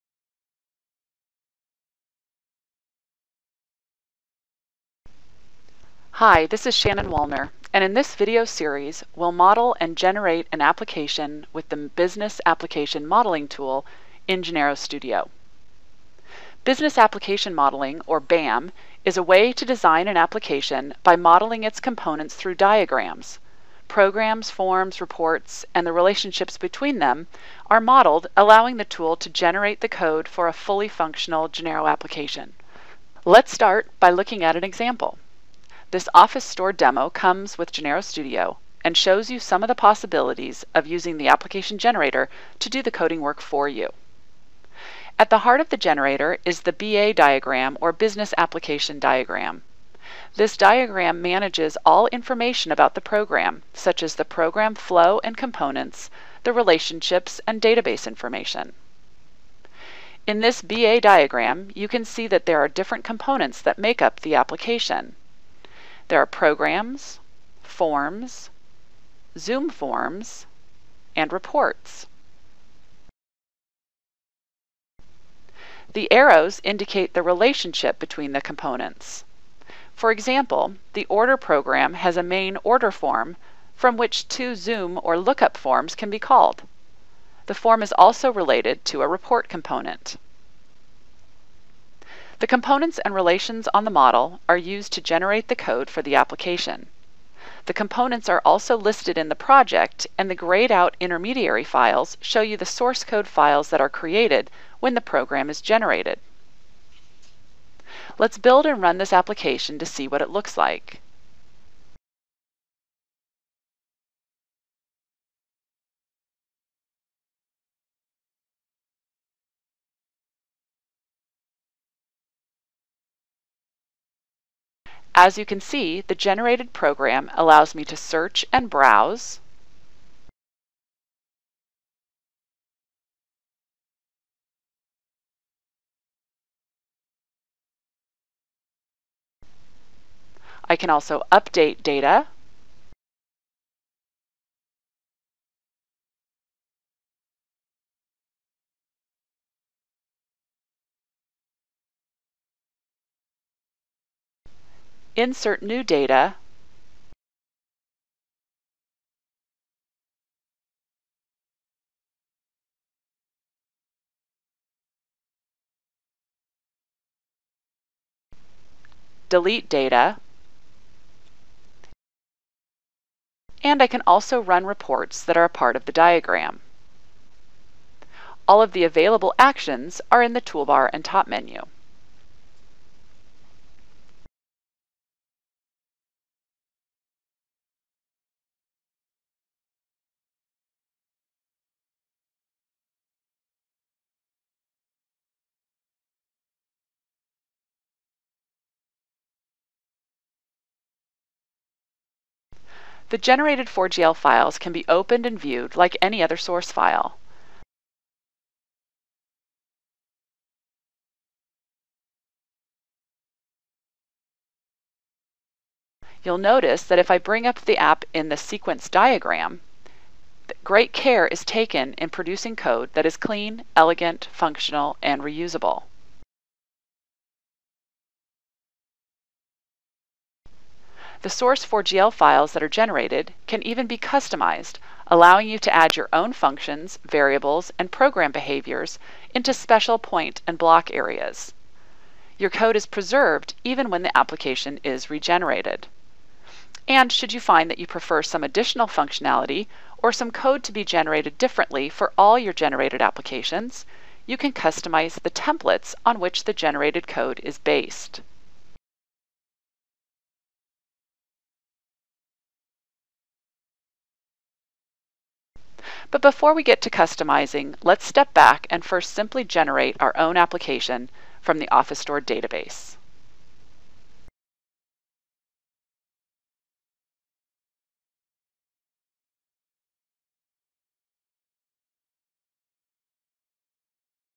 Video 1 of 8: Introduction to Business Application Modeling (BAM)
This video series demonstrates how to model and generate an application with the
business application modeling tool in IBM Informix Genero Studio.